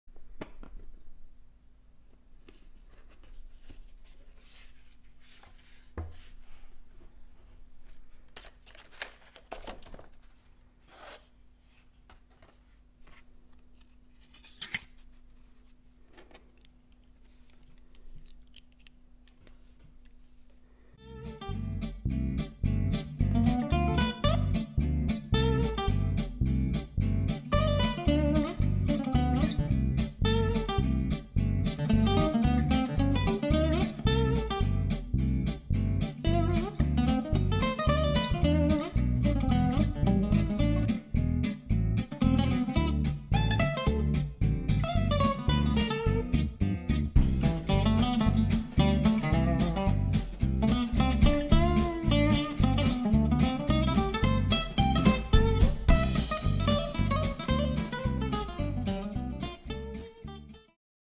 Watercolour acorn
painted the acorn in watercolours - inspired by zaznova_ekaterina